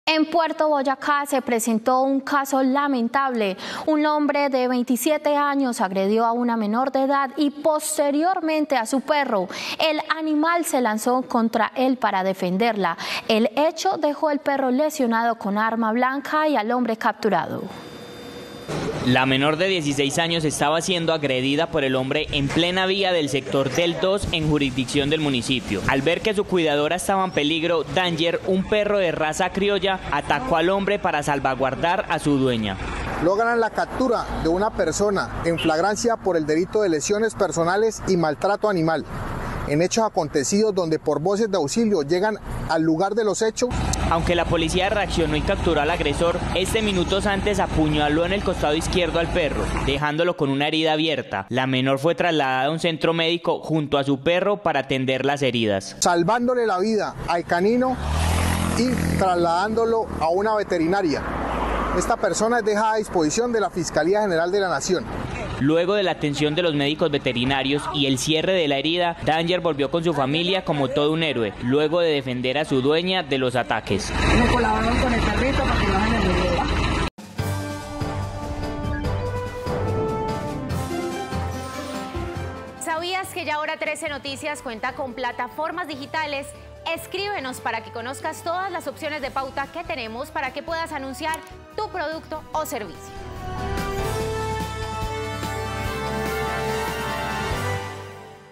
¡Qué valiente! Un perro defendió a una menor de edad que era agredida por un hombre en Puerto Boyacá
El perro fue agredido con arma blanca pero ya está a salvo y el agresor se encuentra en manos de la Fiscalía.